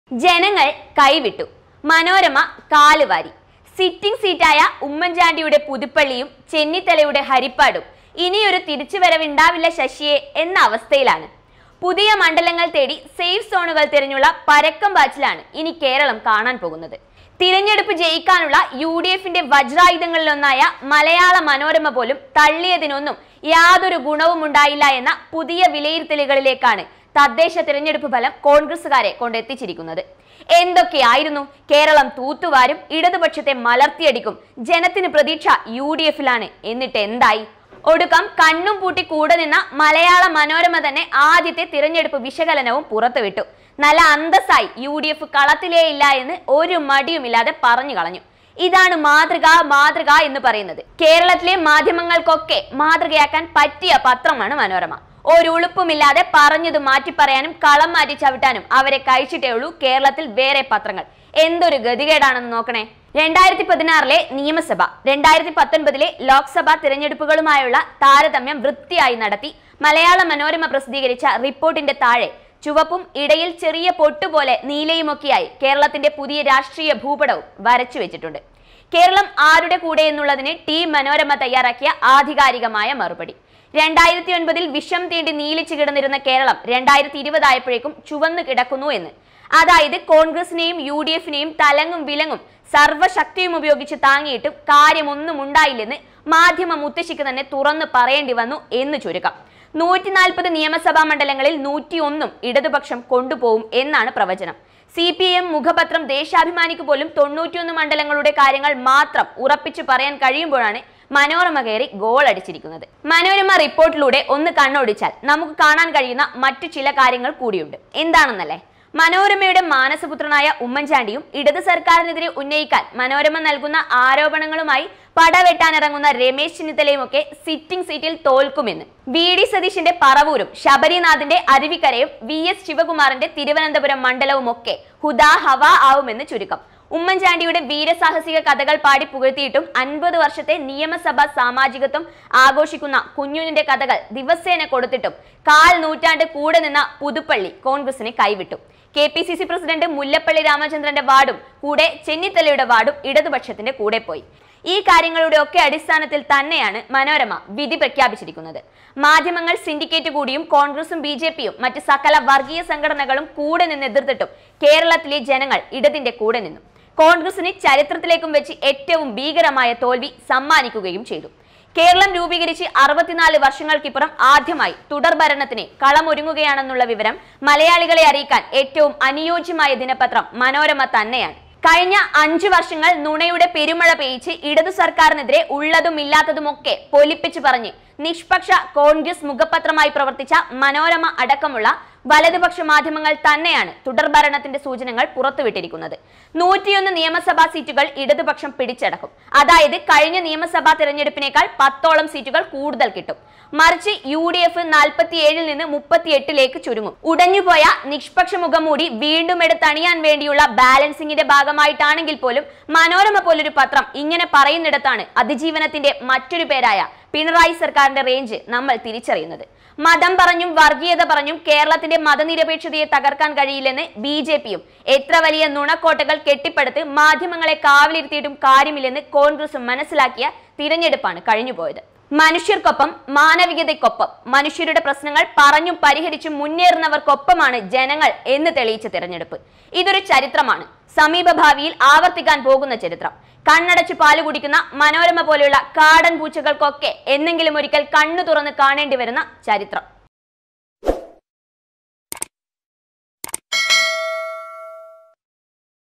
ജനങ്ങൾ കൈവിട്ടു. മനോരമ കാലുവാരി. ചെന്നിത്തലക്കൊപ്പം ചാണ്ടി സാറും തോൽക്കും
നങ്ങൾ കൈവിട്ടു. മനോരമ കാലുവാരി. സിറ്റിംഗ് സീറ്റായ ഉമ്മൻ‌ചാണ്ടിയുടെ പുതുപ്പള്ളിയിലും ചെന്നിത്തലയുടെ ഹരിപ്പാടും ഇനിയൊരു തിരിച്ചുവരവില്ല ശശ്യേ എന്ന അവസ്ഥയിലാണ്. പുതിയ മണ്ഡലങ്ങൾ തേടി സേഫ് സോണുകൾ തിരഞ്ഞുള്ള പരക്കം പാച്ചിലാണ് ഇനി കേരളം കാണാൻ പോകുന്നത്. തിരഞ്ഞെടുപ്പ് ജയിക്കാനുള്ള യൂ ഡി എഫിന്റെ വജ്രായുധങ്ങളിൽ ഒന്നായ മലയാള മനോരമ പോലും തള്ളിയതിനൊന്നും യാതൊരു ഗുണവുമുണ്ടായില്ല എന്ന പുതിയ വിലയിരുത്തലിലേക്കാണ് തദ്ദേശ തിരഞ്ഞെടുപ്പ് ഫലം കോൺഗ്രെസ്സുകാരെ കൊണ്ടെത്തിച്ചിരിക്കുന്നത്. എന്തൊക്കെയായിരുന്നു. കേരളം തൂത്തുവാരും. ഇടതുപക്ഷത്തെ മലർത്തിയടിക്കും. ജനത്തിന് പ്രതീക്ഷ യൂ ഡി എഫിലാണ്. എന്നിട്ടെന്തായി? ഒടുക്കം കണ്ണും പൂട്ടി കൂടെ നിന്ന മലയാള മനോരമ തന്നെ ആദ്യത്തെ തിരഞ്ഞെടുപ്പ് വിശകലനം പുറത്തുവിട്ടു. നല്ല അന്തസ്സായി യൂ ഡി എഫ് കളത്തിലെ ഇല്ല എന്ന് ഒരു മടിയുമില്ലാതെ പറഞ്ഞു കളഞ്ഞു. ഇതാണ് മാതൃക മാതൃക എന്ന് പറയുന്നത്. കേരളത്തിലെ മാധ്യമങ്ങൾക്കൊക്കെ മാതൃകയാക്കാൻ പറ്റിയ മാധ്യമമാണ് മനോരമ. ഒരു ഉളുപ്പുമില്ലാതെ പറഞ്ഞത് മാറ്റിപ്പറയാനും കളം മാറ്റി ചവിട്ടാനും അവരെ കഴിച്ചിട്ടേ ഉള്ളു കേരളത്തിൽ വേറെ പത്രങ്ങൾ. എന്തൊരു ഗതികേടാണെന്നു നോക്കണേ @TheKeralaObserver

ഞങ്ങളുടെ വാട്സാപ്പ് ഗ്രൂപ്പ്. Please join our Whatsapp group.